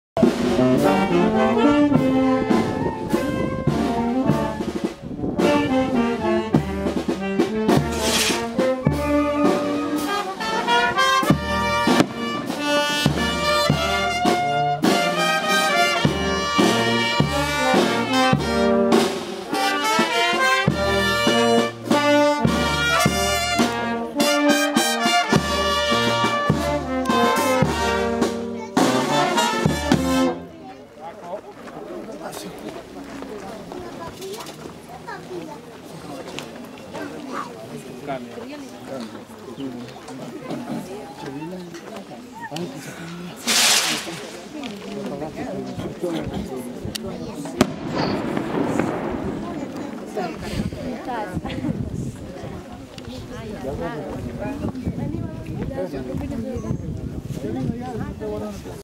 COTO - HUARAL - FIESTA 2012
Tradicional fiesta en honor a la Virgen del Carmen en el hermoso Pueblo de Coto - Huaral, los días 2,3 y 4 de Agosto. Estamos observando la procesión después de la Santa Misa.